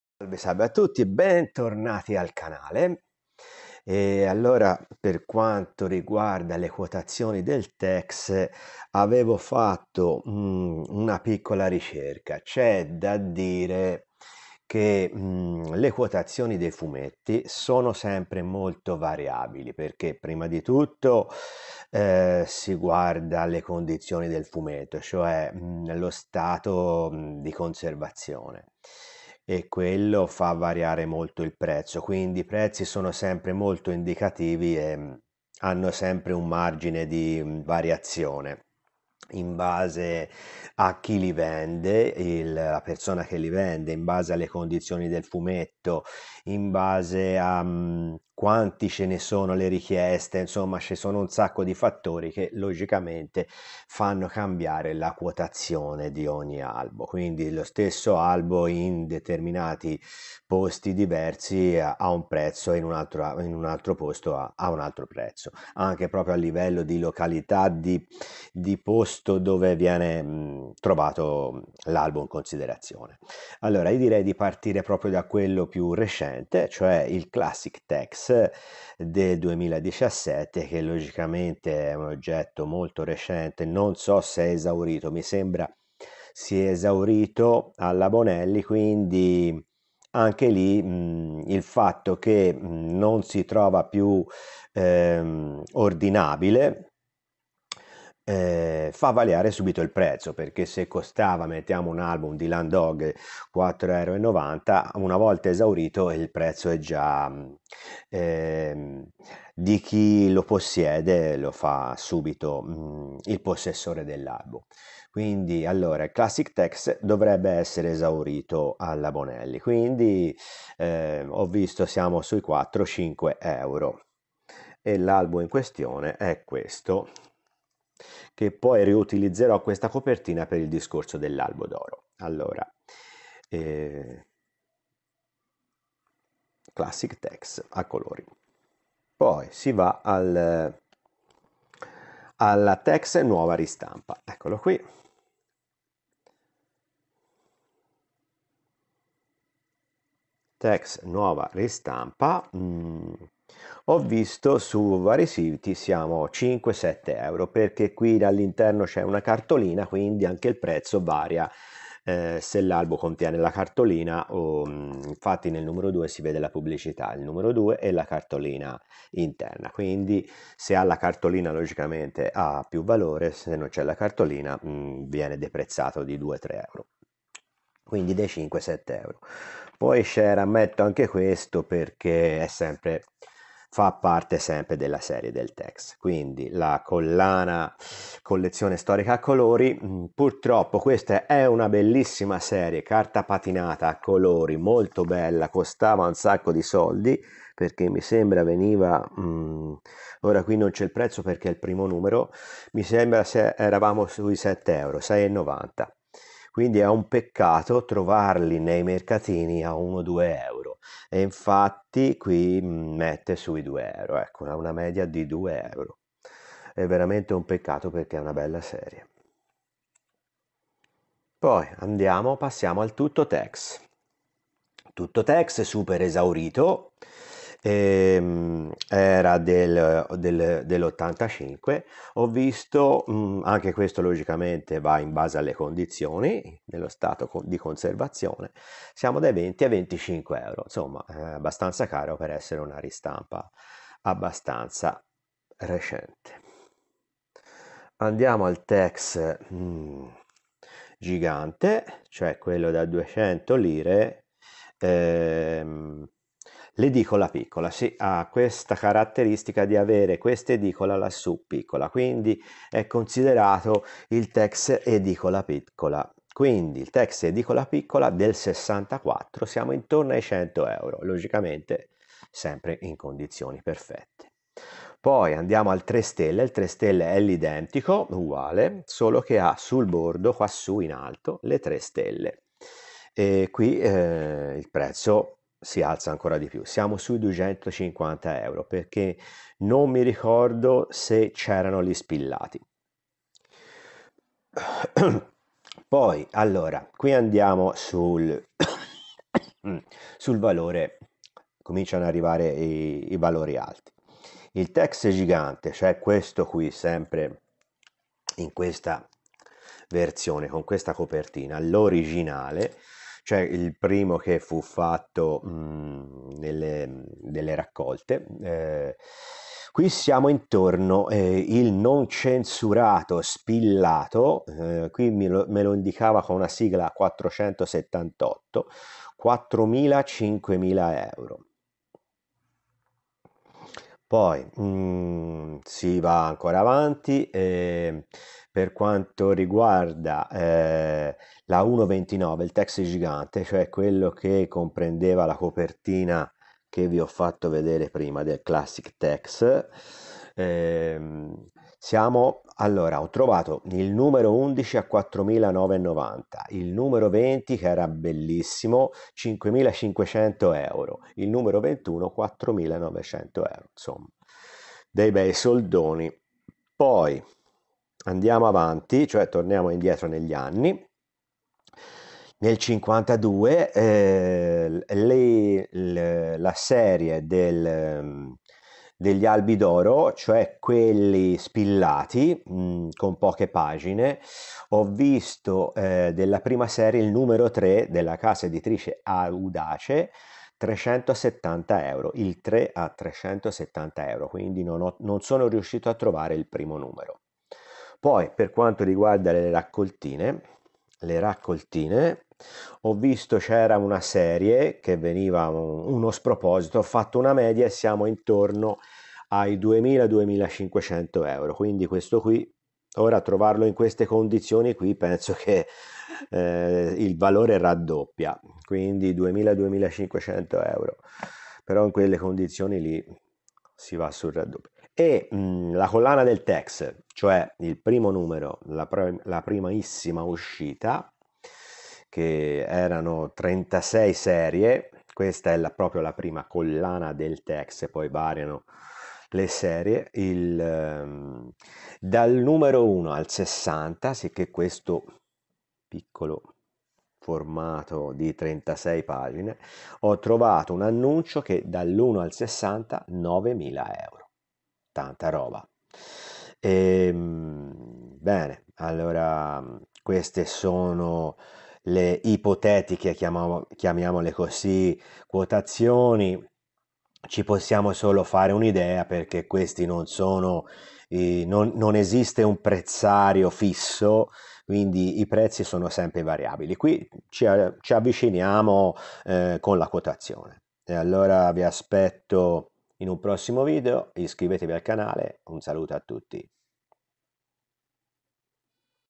TEX terza parte quotazioni
Ricerche valutazioni e prezzi sui vari siti ed annunci collezionistici.